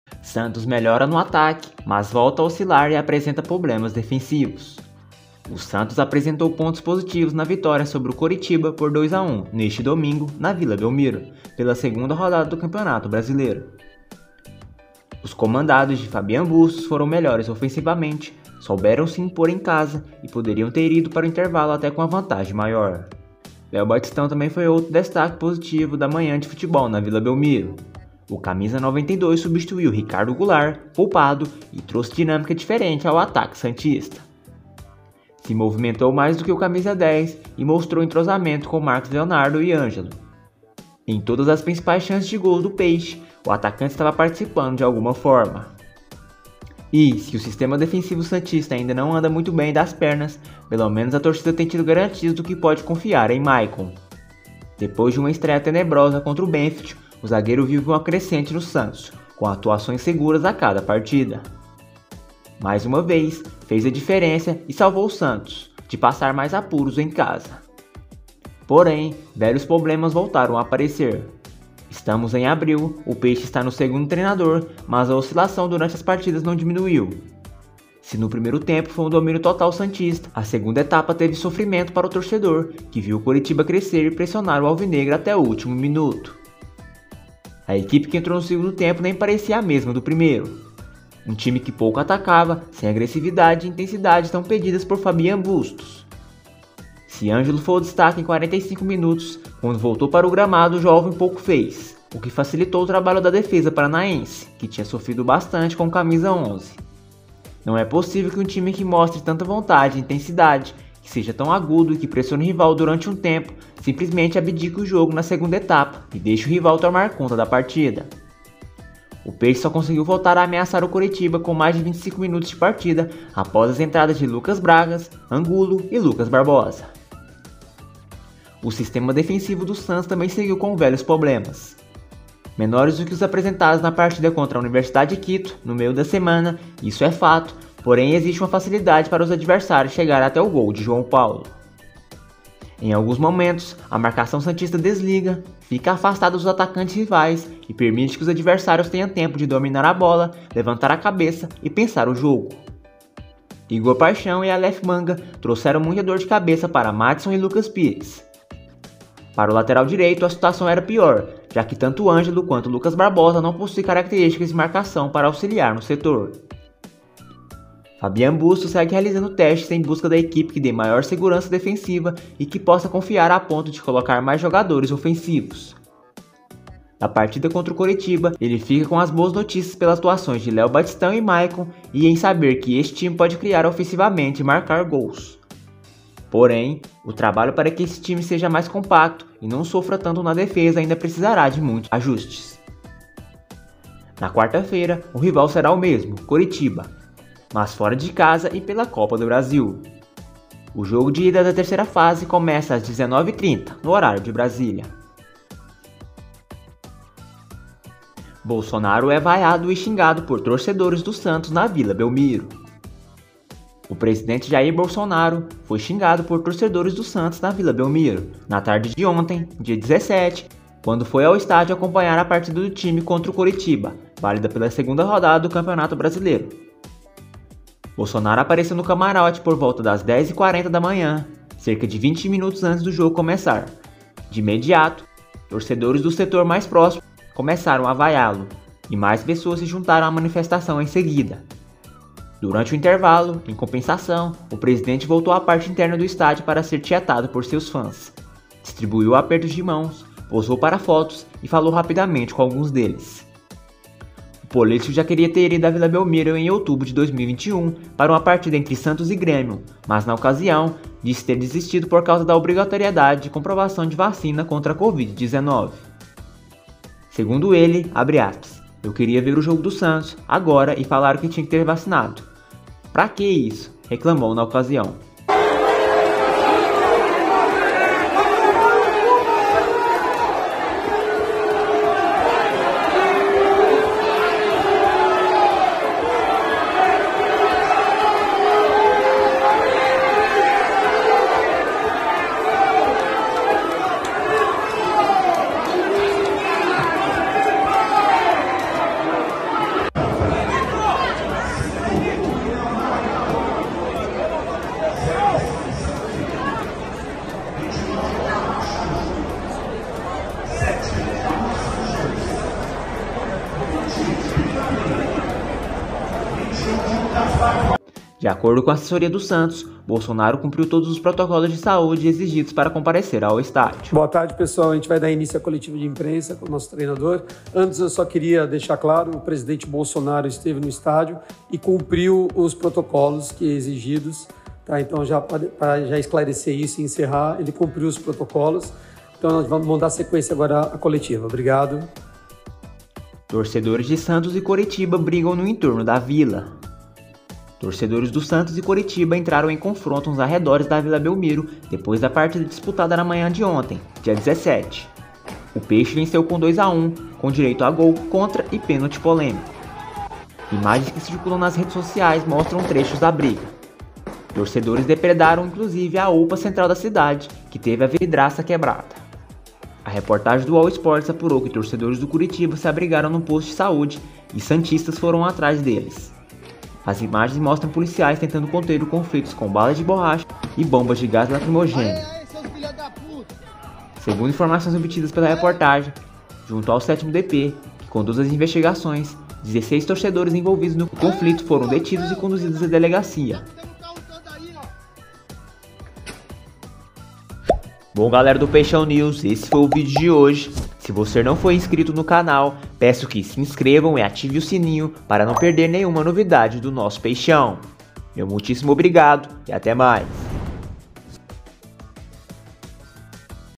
🔥🤯 PÓS JOGO DE VITÓRIA DO SANTOS NO BRASILEIRÃO! VAIAS A BOLSONARO NA VILA E BRIGA DE TORCEDORES!
PÓS JOGO DE VITÓRIA DO SANTOS NO BRASILEIRÃO POR 2X1 CORITIBA! BOLSONARO É MUITO VAIADO NA VILA BELMIRO E BRIGA DE TORCEDORES AO REDOR DO ESTÁDIO!